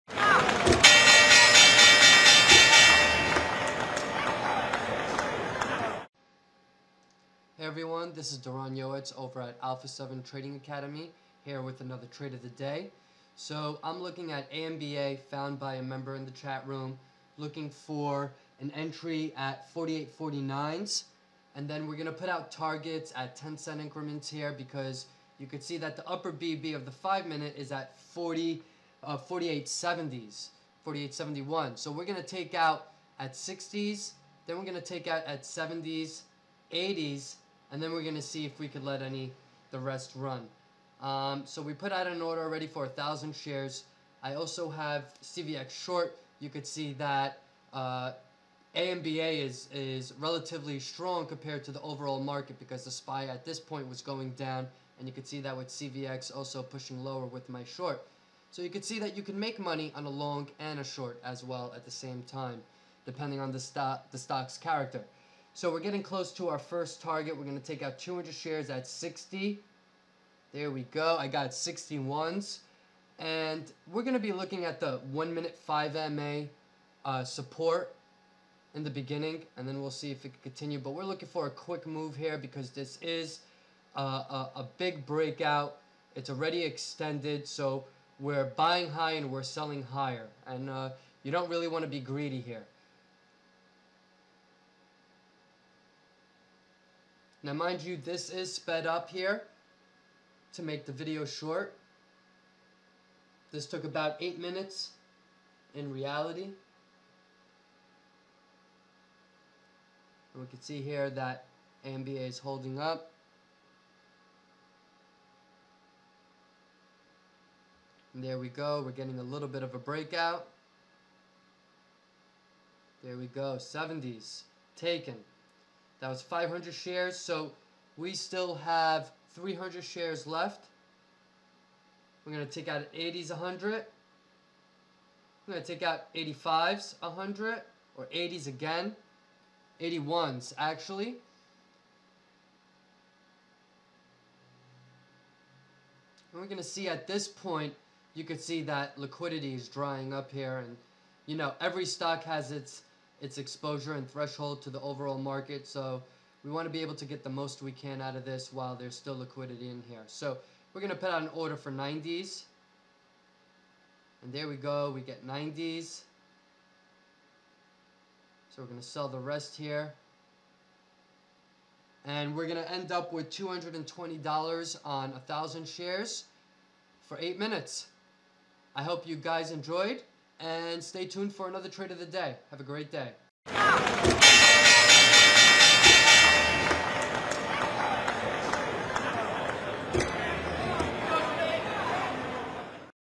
AMBA Perfect Storm Breakout - Flawless Trading
3 multiple set-ups simultaneously breakout at the same time causing a high probability set up to be unleashed!

Alpha7Trading.com